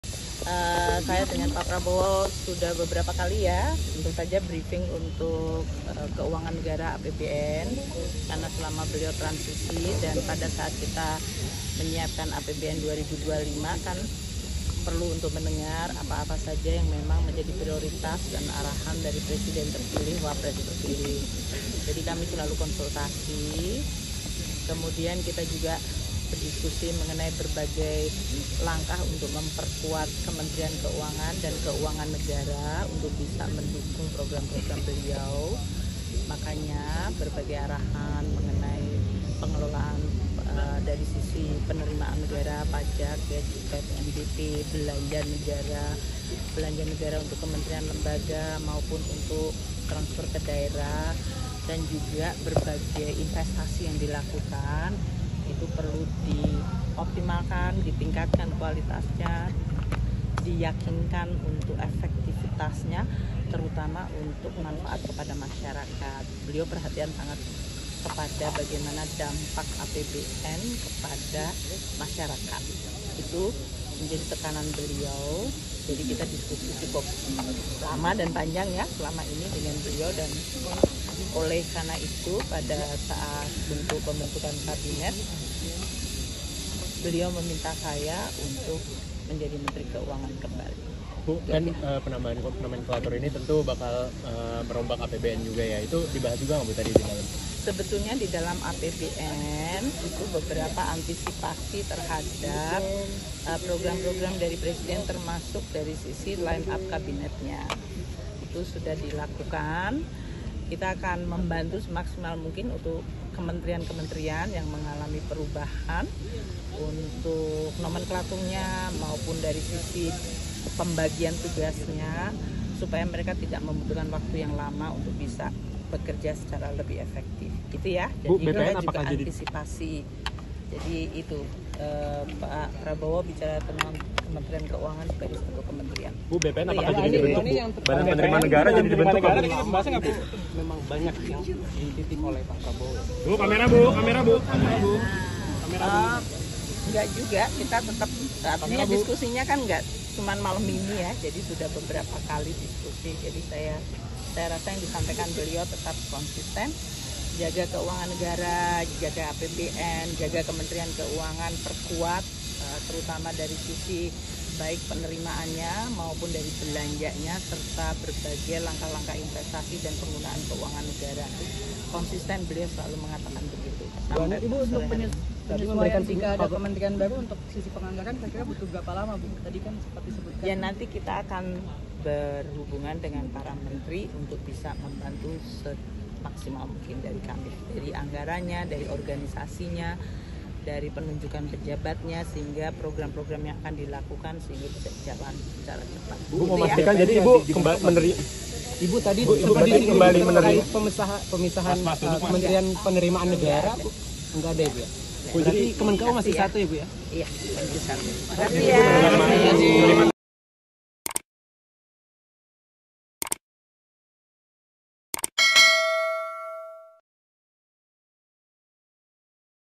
Bertemu Prabowo, Sri Mulyani Diminta Jadi Menteri Keuangan Lagi
Menteri Keuangan era Jokowi-Maruf Amin, Sri Mulyani, mengaku diminta oleh presiden terpilih Prabowo Subianto untuk menjadi Menteri Keuangan di kabinetnya.
Sri Mulyani menyampaikan hal itu setelah bertemu presiden terpilih di kediaman Prabowo, Jalan Kertanegara, Jakarta Selatan, Senin (14/10/2024).
Sri Mulyani menyebut bahwa dia dan Prabowo sudah berdiskusi mengenai pengelolaan penerimaan negara, seperti pajak dan penerimaan negara bukan pajak (PNBP).
Mereka juga membahas soal belanja negara untuk kementerian/lembaga maupun transfer ke daerah.
Simak videonya berikut ini.
Video Jurnalis: Dimas Nanda KrisnaPenulis Naskah: Dimas Nanda KrisnaVideo Editor: Dimas Nanda KrisnaProduser: Nursita Sari